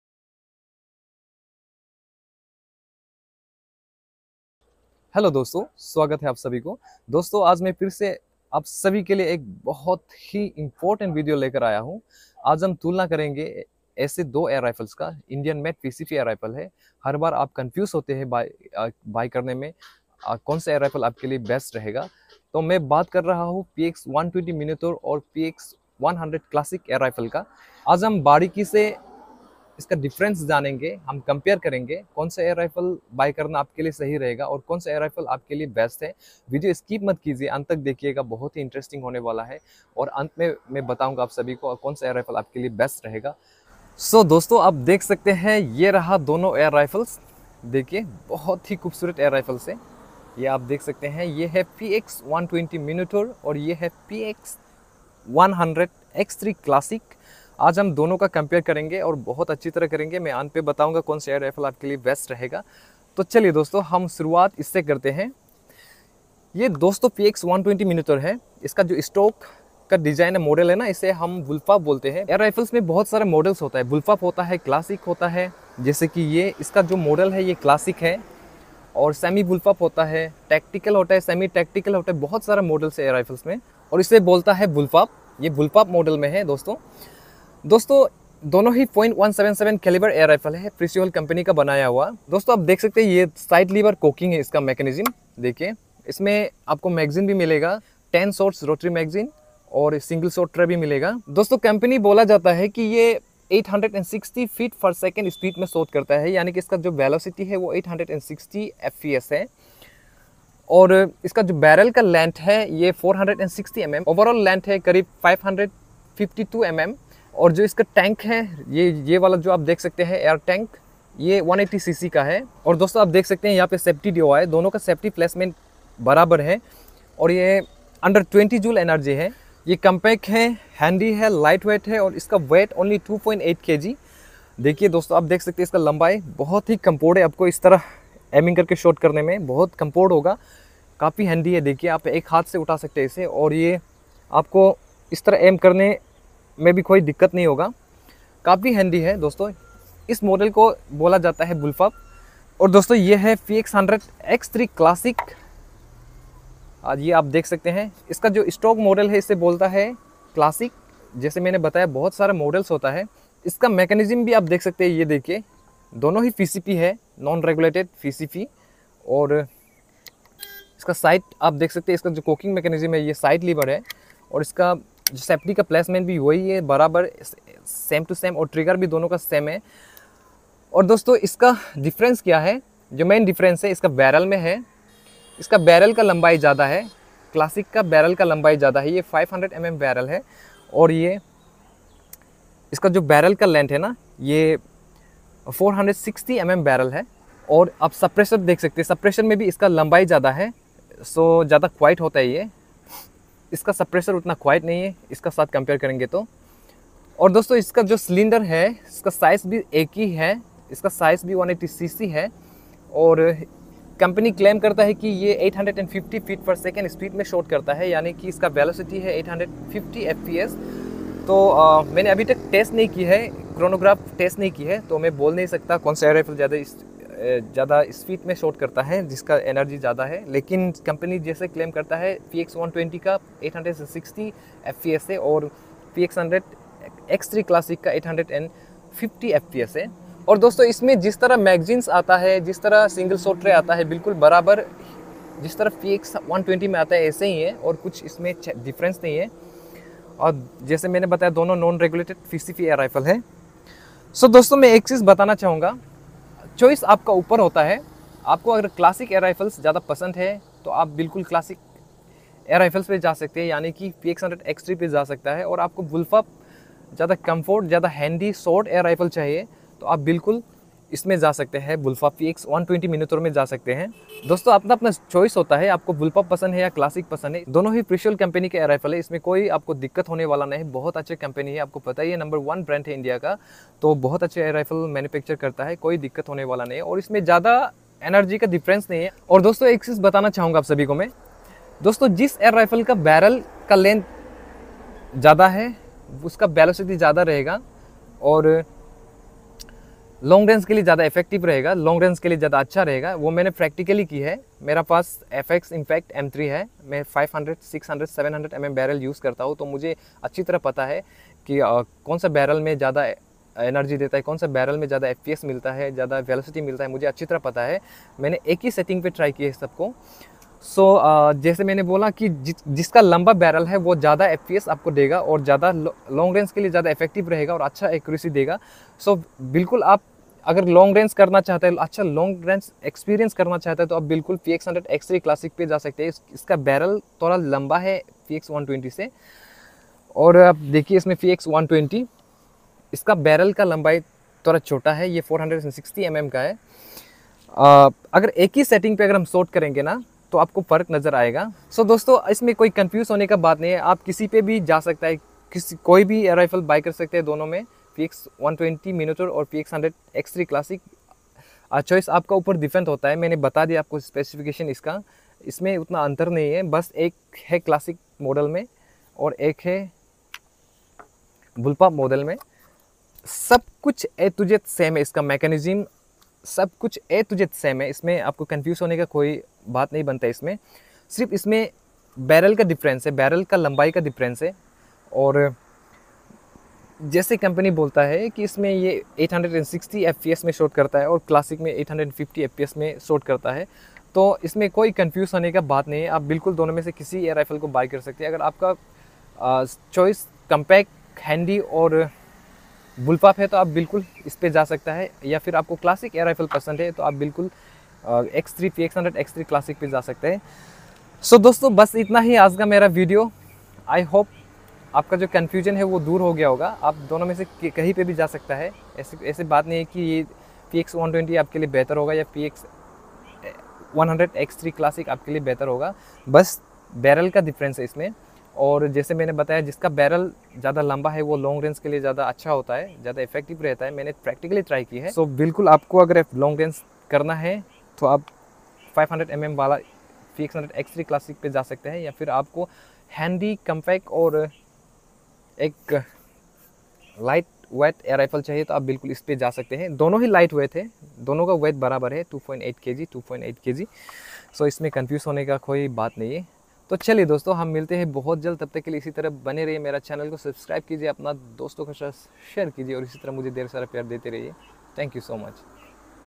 Comparison video/ Px120 Minotaur & px100 x3 classic/choice the right air rifle according to ur need.
The air rifles shown  in the video are India made pcp px120 Minotaur & px100 x3 classic. Both are .177 and under 20 joules energy which doesn't require fire arms license to shot/own as per arm act 2016.